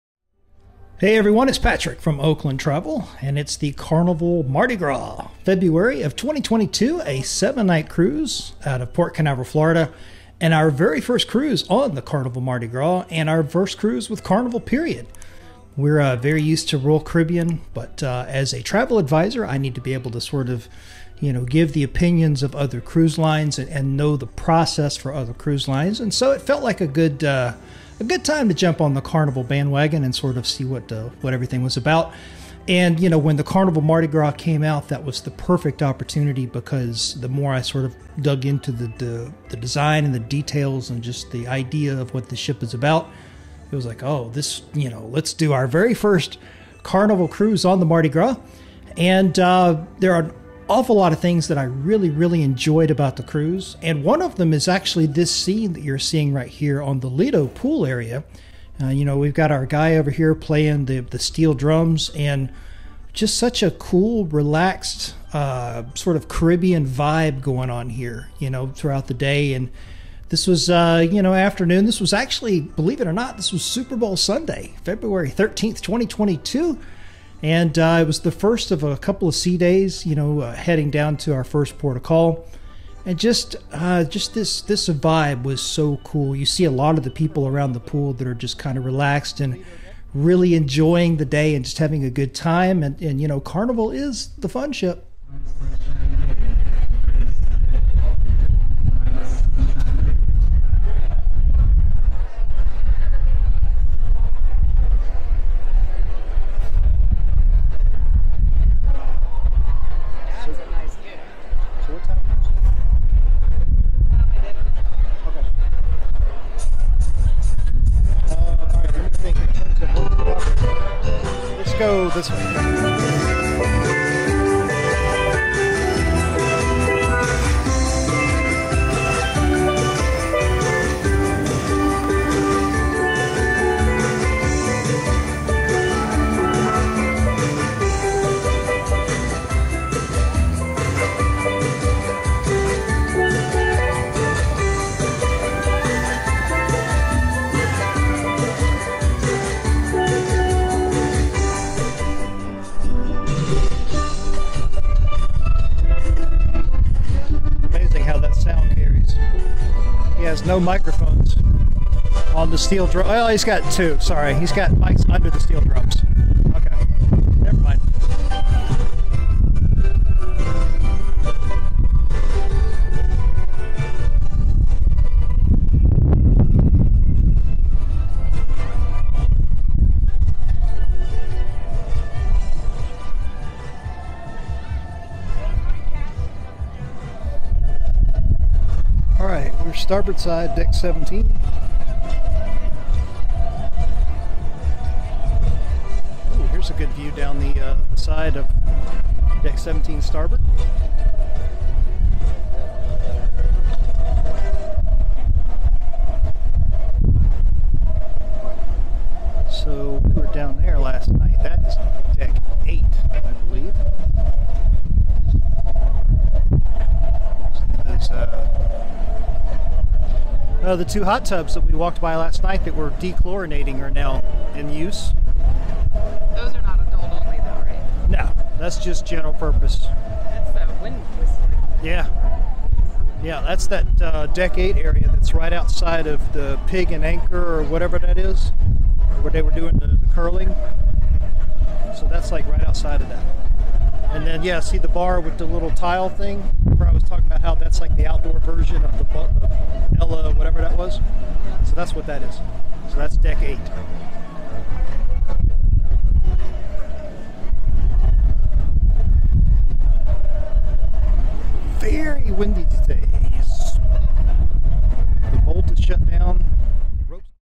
Carnival Mardi Gras - Steel Drums | Oakland Travel
On a cruise, nothing is more relaxing than laying out by the pool on a perfect day with a light breeze and some Caribbean music with steel drums playing in the background. In episode 38 of the vlog from our February 2022 cruise on the Carnival Mardi Gras, we're enjoying a relaxing day at sea by the Lido pool.